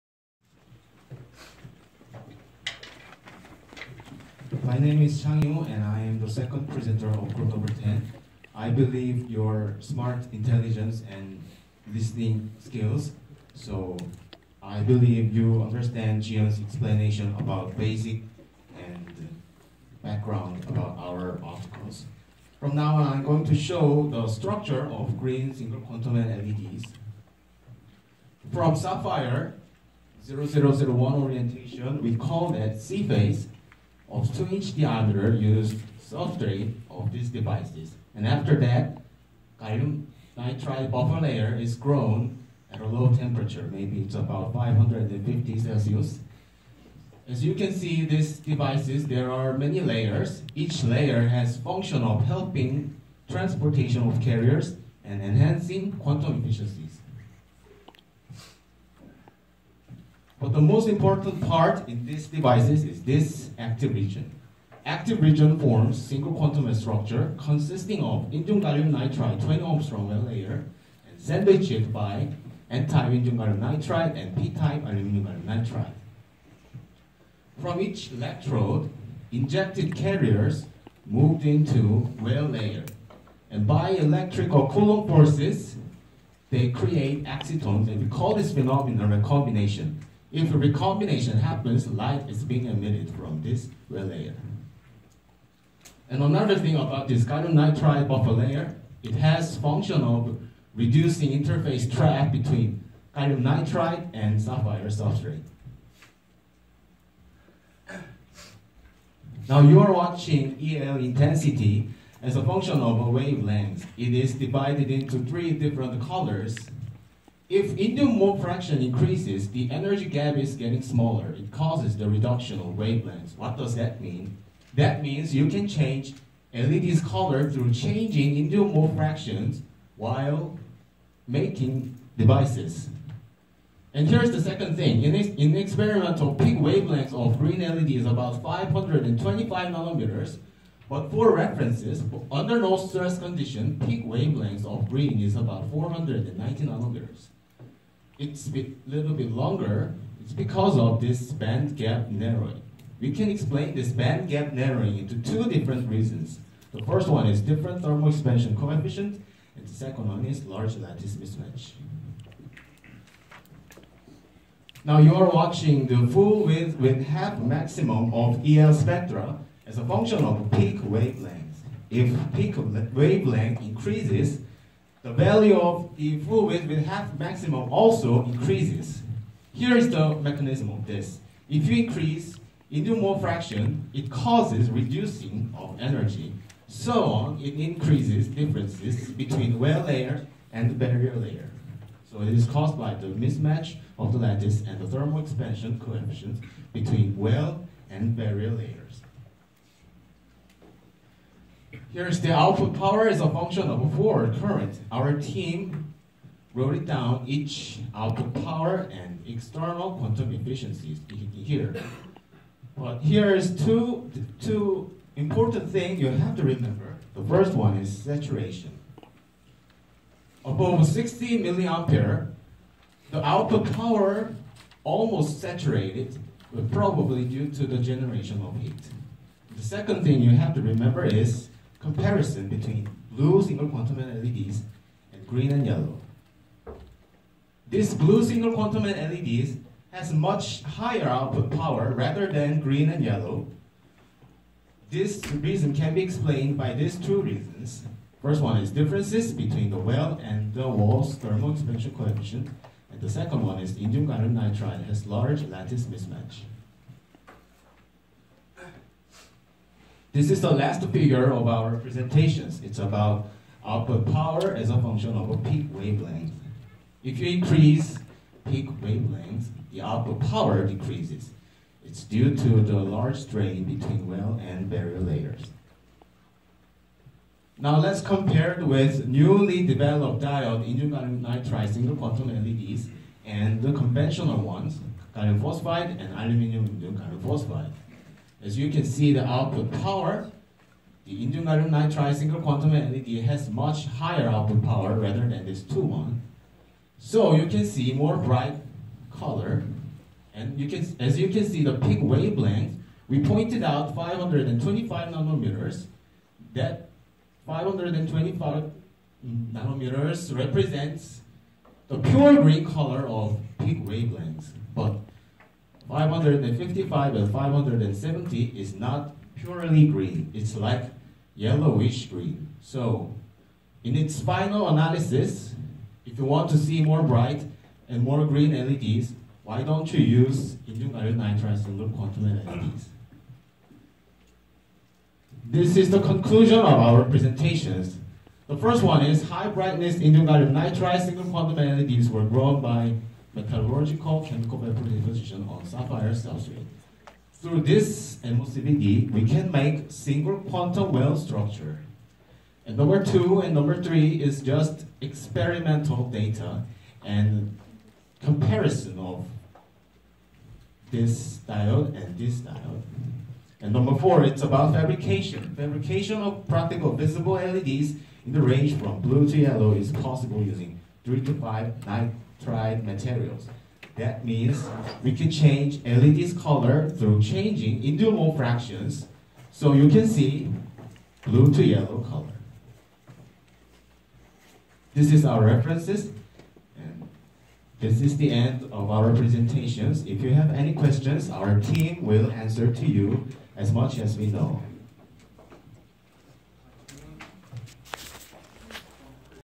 light-emitting diodes with quantum well structure(InGaN SQW LED)
Speaker : Chang hee woo, KW university

About reference,
Article : High-Brightness InGaN Blue, Green and Yellow Light-Emitting Diodes with Quantum Well Structures
Author : Shuji Nakamura, Masayuki Senoh, Naruhito Iwasa and Shin-ichi Nagahama

양자 우물 구조를 가진 발광다이오드 관련 프리젠테이션입니다.
소자에 대한 전반적인 구조, 본 논문에 나와있는 figure분석 및 기존 다이오드와 SQW led와의 비교를 통해서 알아보았습니다.